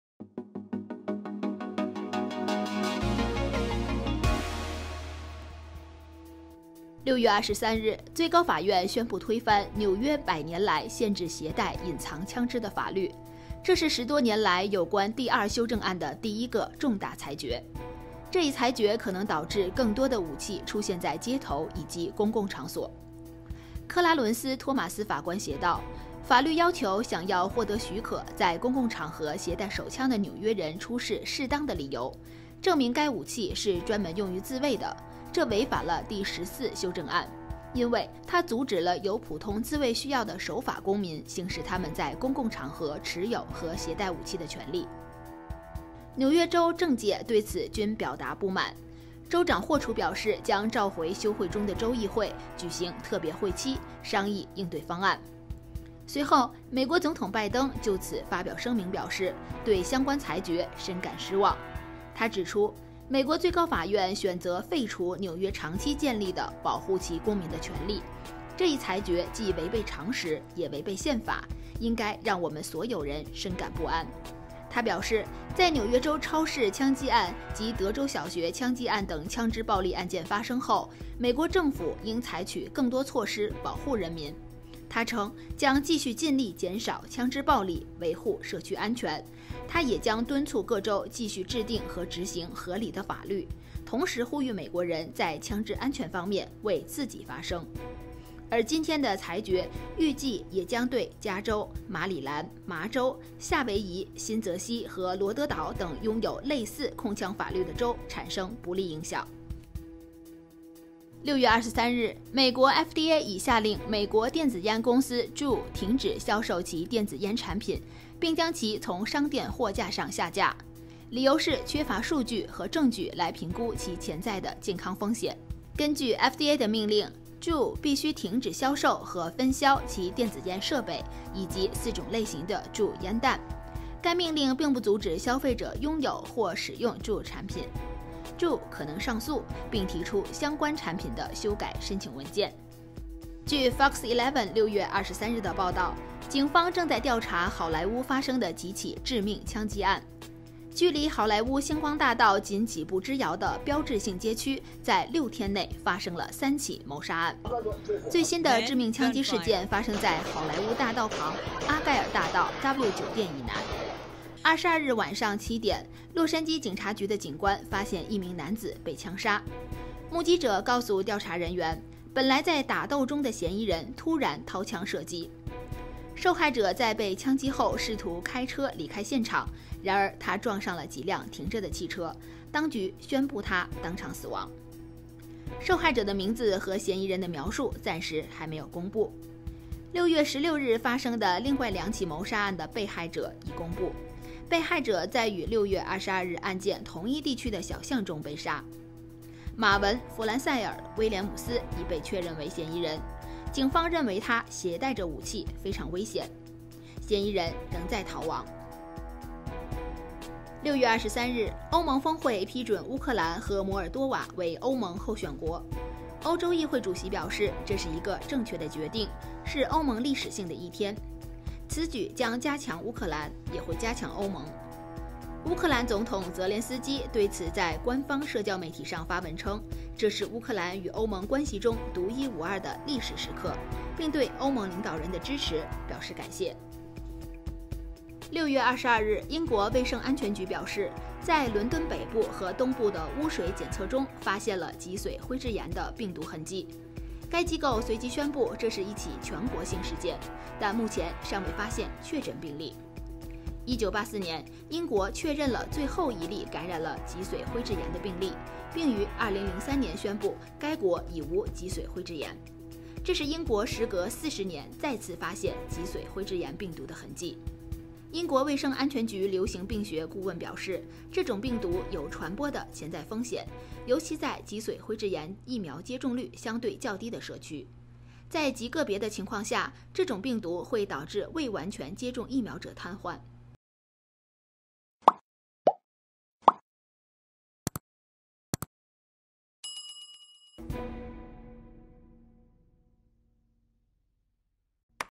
美最高法推翻纽约州禁止随身带枪的法律，美FDA宣布禁销JUUL电子烟
00:00  片头

00:06  最高法推翻纽约携带隐蔽武器的法律

01:51  美FDA宣布禁销JUUL电子烟

02:30  好莱坞闹市区连续发生枪击案 嫌犯在逃

03:37  乌克兰获得欧盟候选国地位

04:05   40年来首次！英国生活污水中检测出脊髓灰质炎病毒